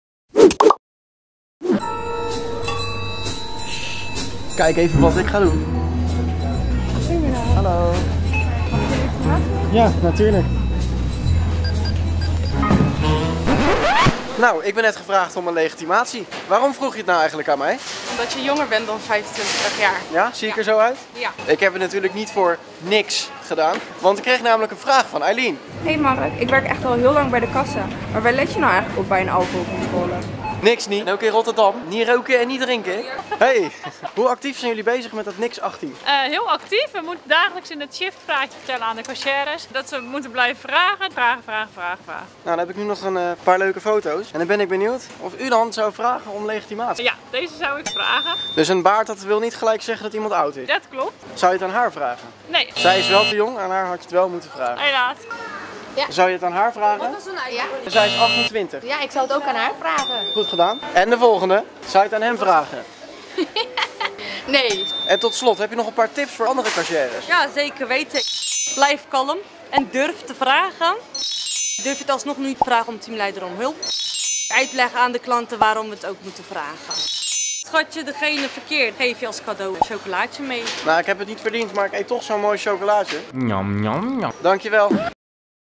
Twijfel: wel of niet om ID vragen? | De Boodschapper Vlog #11
Je kent het wel: de twijfel of je wel of niet om ID moet vragen. Je leidinggevende helpt je er graag bij! Mark trouwens ook. ;-) Hoe goed ben jij in leeftijden inschatten?